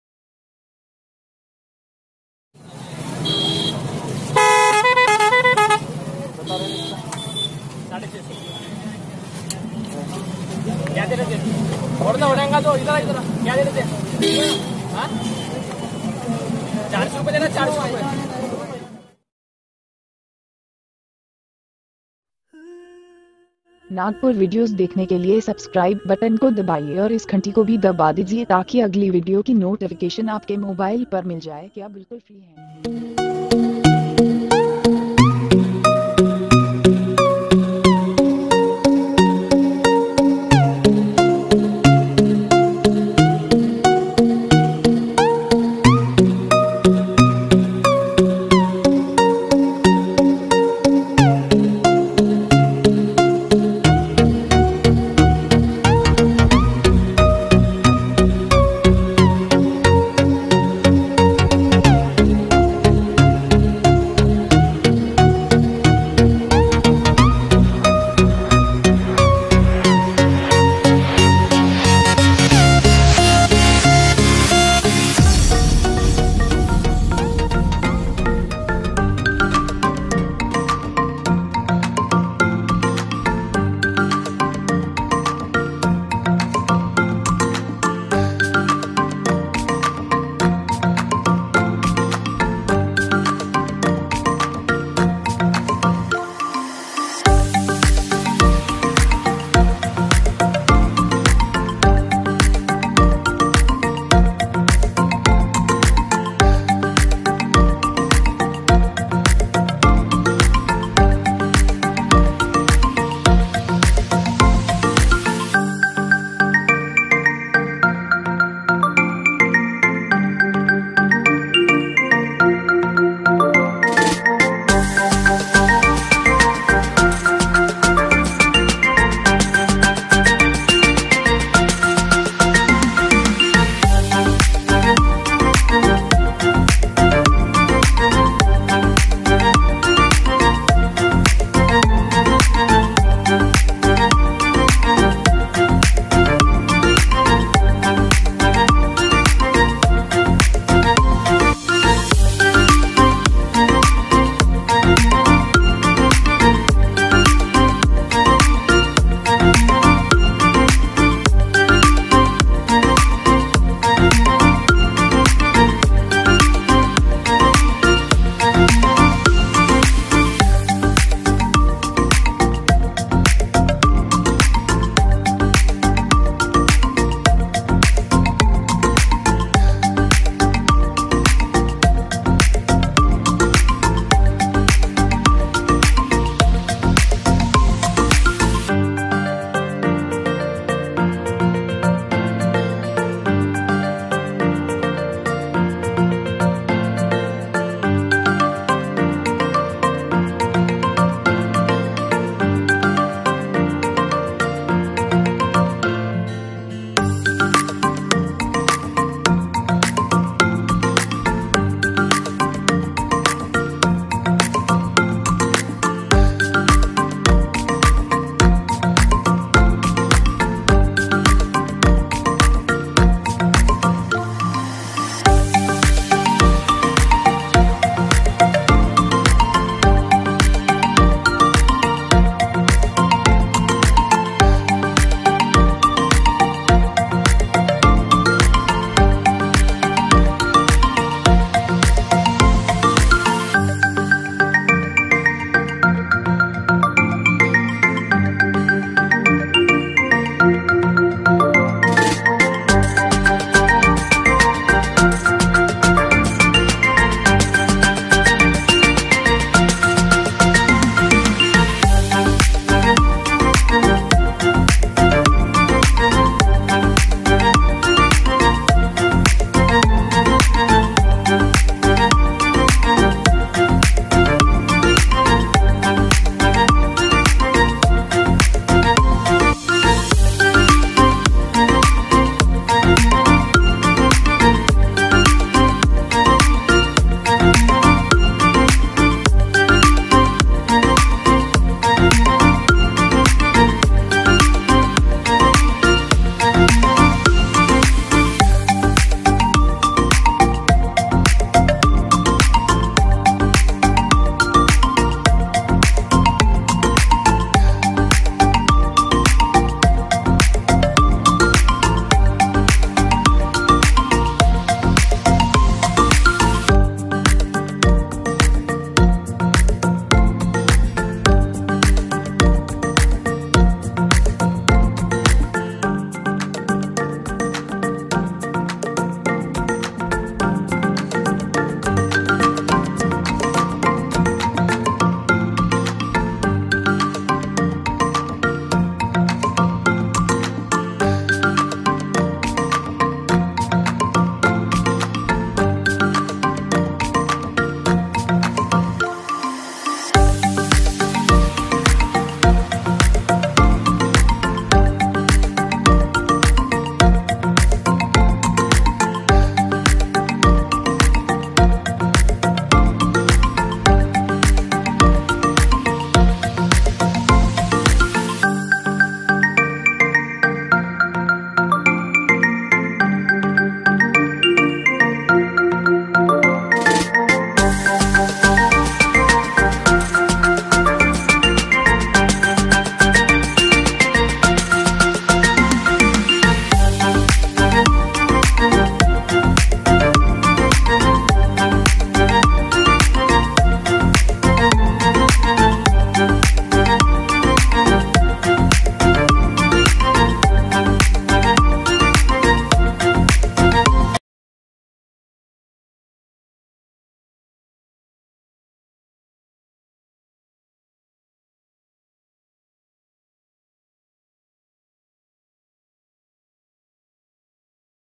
Chor bazar nagpur 24/10/ 2020, चोर बाजार नागपूर महाराष्ट
नागपुर चोर बाजार लॉकडाउन के बाद काफी महीनो बाद फिर पहले जैसा शुरू हो गया है इस वीडियो में देखिये चोर बाजार   नागपुर का यह २४/१०/२०२० का लेटेस्ट वीडियो  नागपुर के इस चोर बाजार में पुराने मोबाइल , लैपटॉप, गाड़ियों के पार्ट्स ,जिम का सामान ,और काफी सामान अच्छे दाम  में मिलता है। चोर बाजार हर शनिवार को लगता है इसे शनिवार बाजार भी कहा जाता है।   सेकंड हैंड वस्तुओ के लिए यह नागपुर मार्केट  फेमस है।